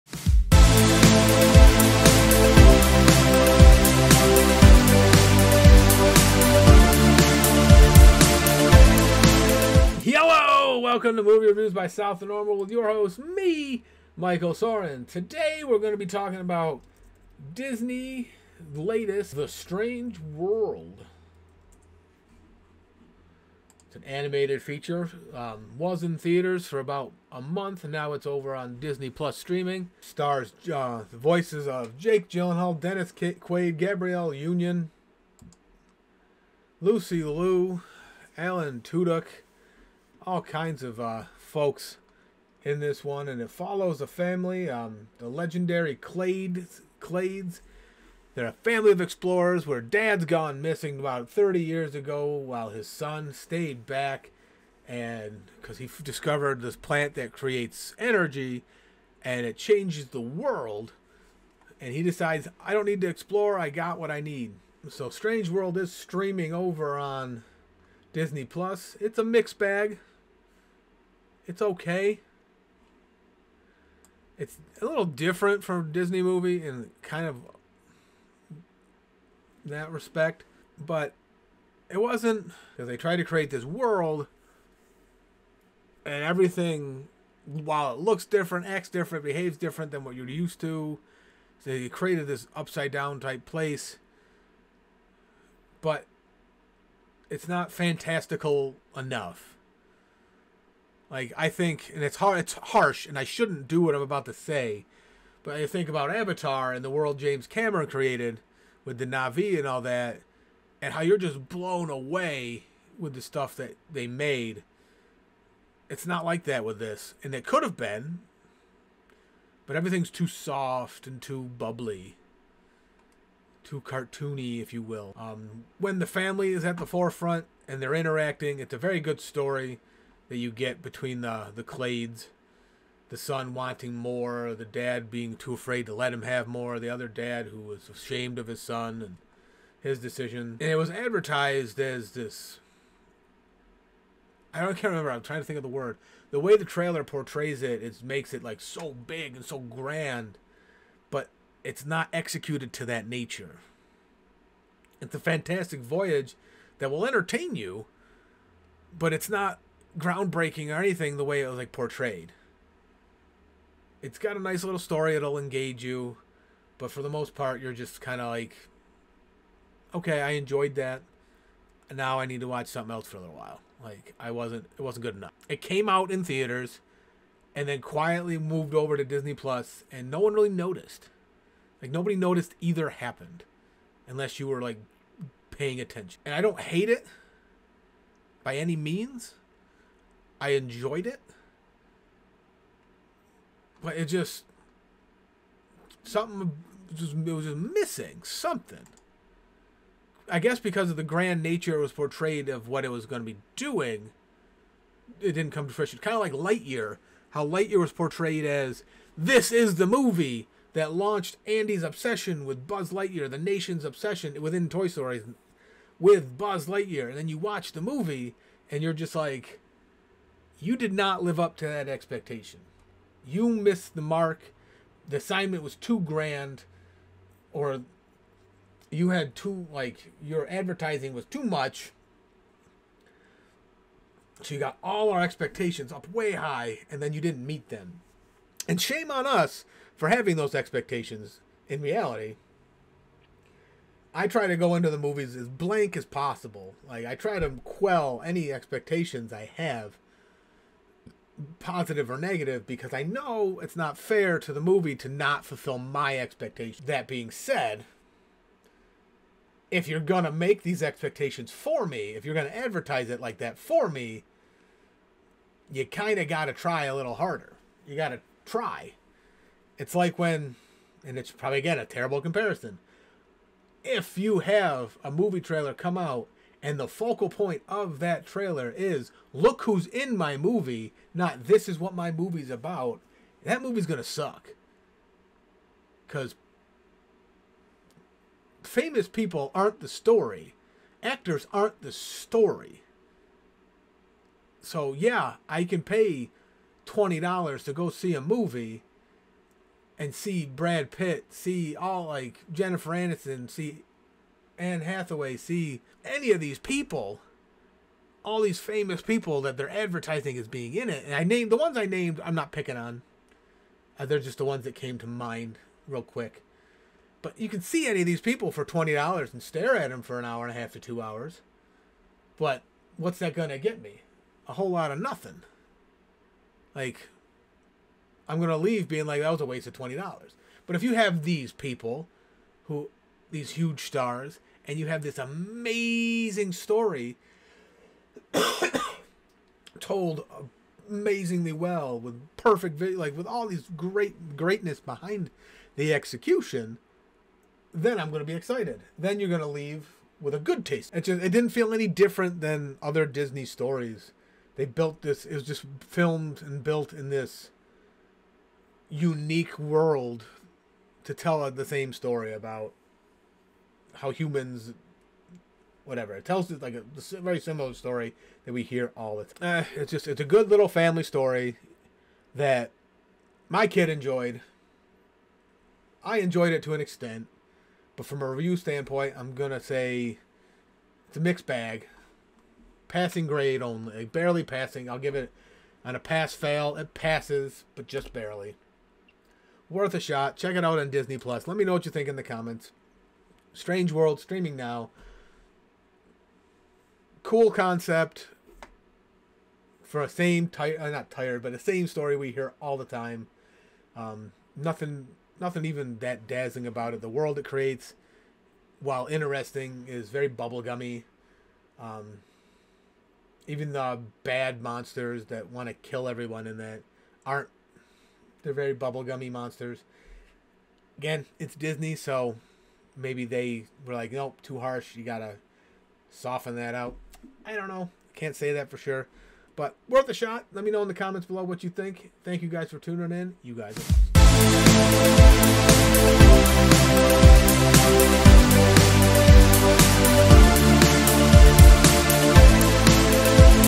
Strange World Review - Disney
The legendary Clades are a family of explorers whose differences threaten to topple their latest and most crucial mission.
Tune in to hear my take!